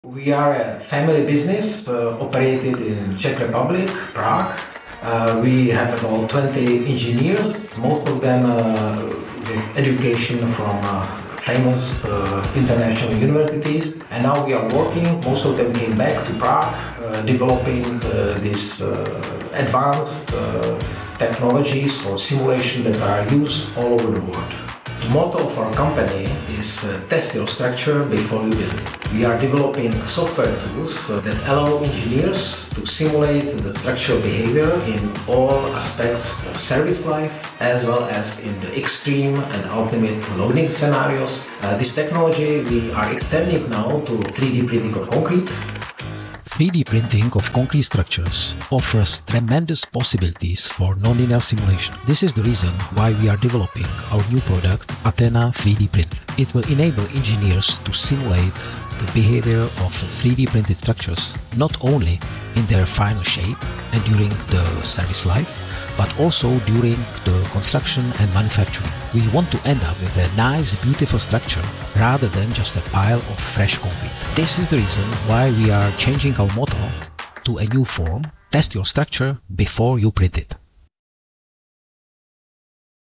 Simulation of 3D printing of concrete in ATENA software
Our motivation for the development of 3D concrete printing simulation services.